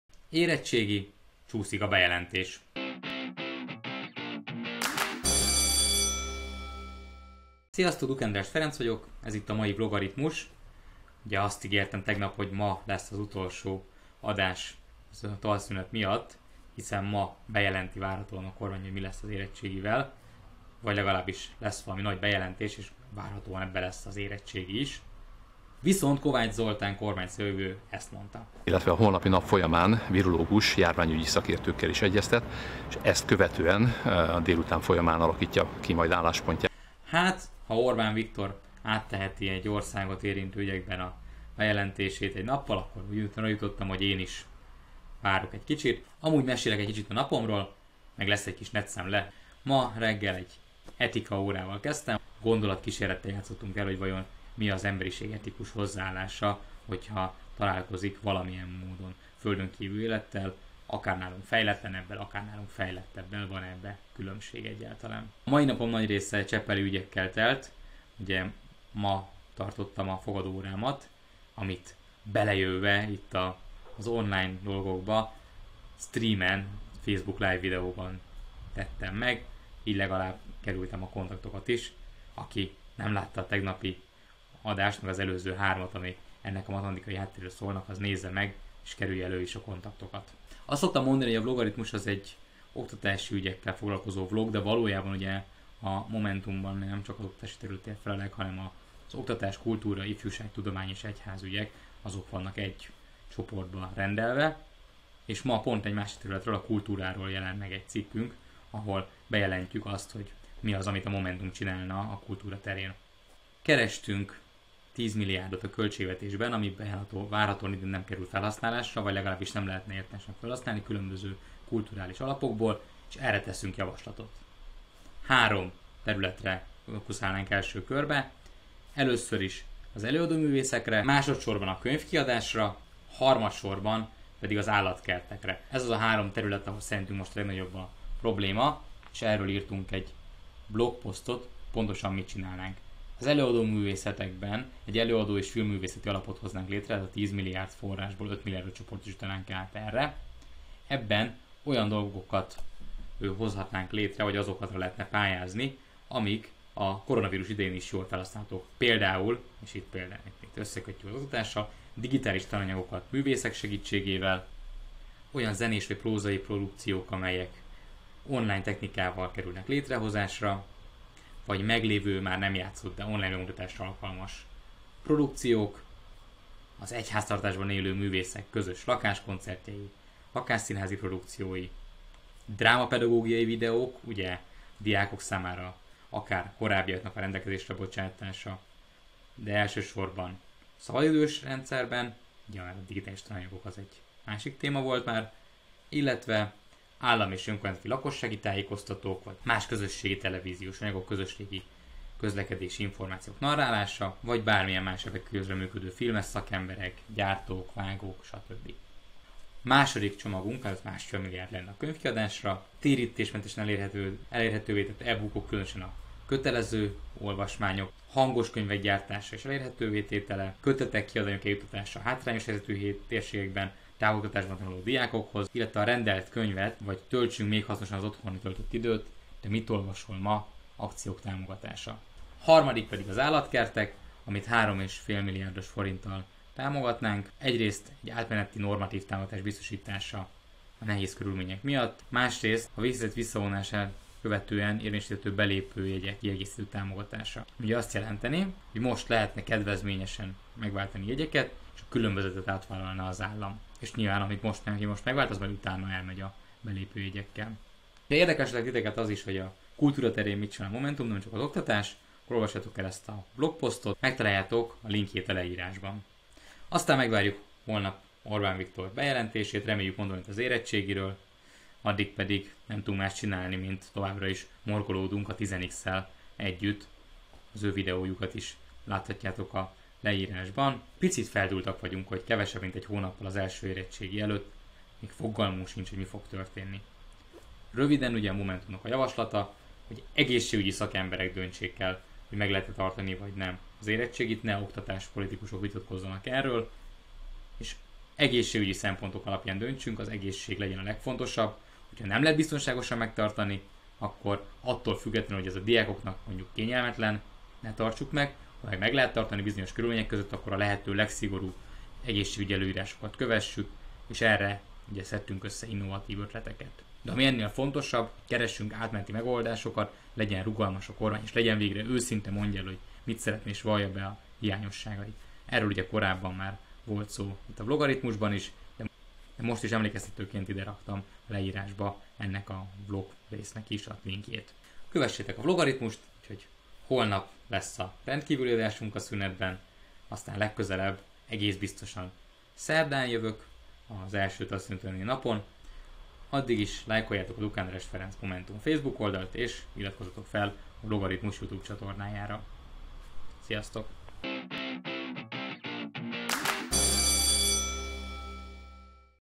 Vlogaritmus 20. rész: Kulturális javaslataink
Mivel az érettségivel bejelentés ma nem történt meg, ezért ma a kultúrpolitikai javaslatcsomagunkról mesélek. (Meg egy kicsit a mai etika órámról meg Csepelről.)

Várjuk azért továbbra is az érettségiről a döntést, a mi javaslataink ebben a Vlogaritmus részben vannak